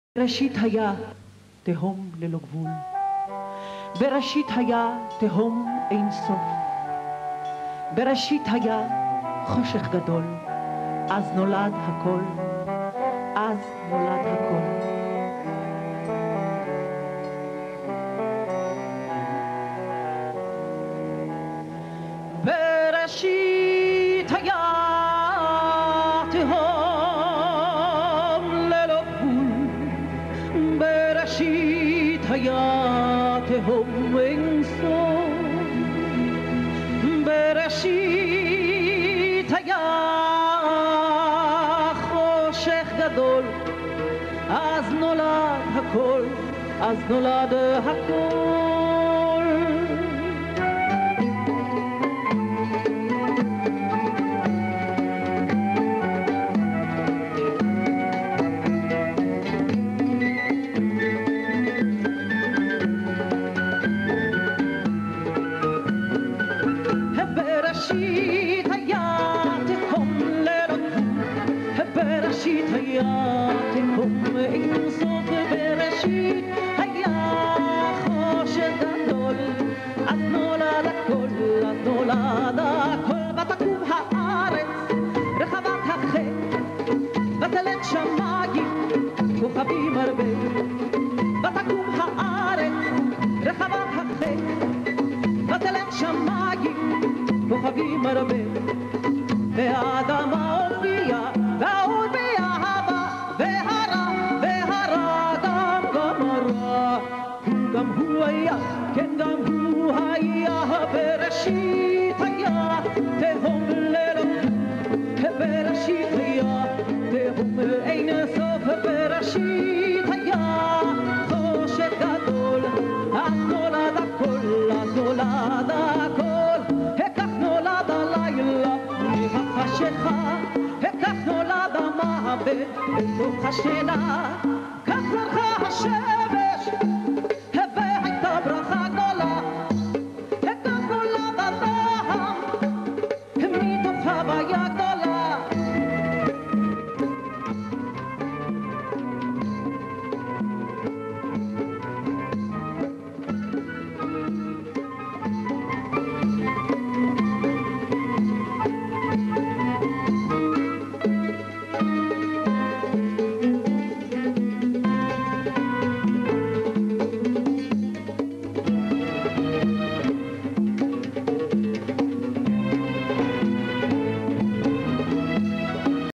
מוסיקת עולם/ קאיה בראשית.wmv
מתוך מופע מקורי כתבה קאיה סיפורי האלים במיתולוגיה היוונית